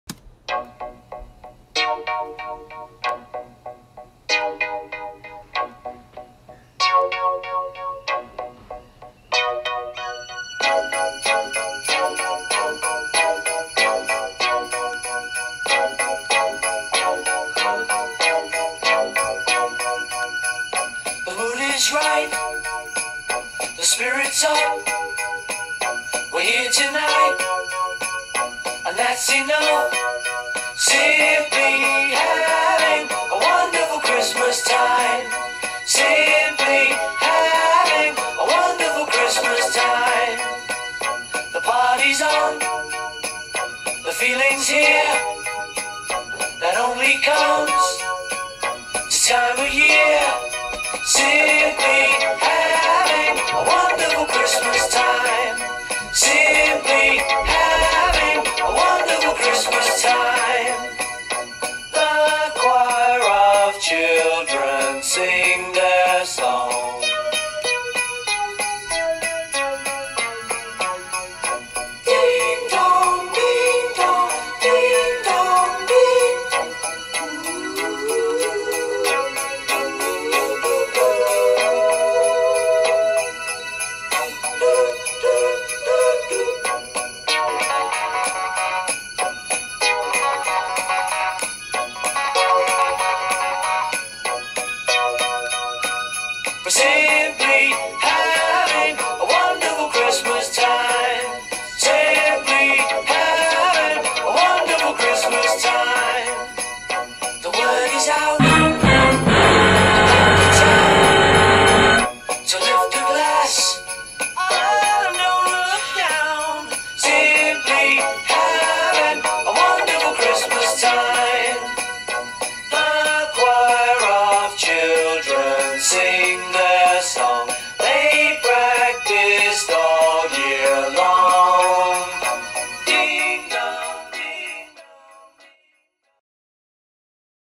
Top 5 Best Christmas Movies
Merry christmas and a happy new year. Watch our opinion of the top 5 best christmas movies.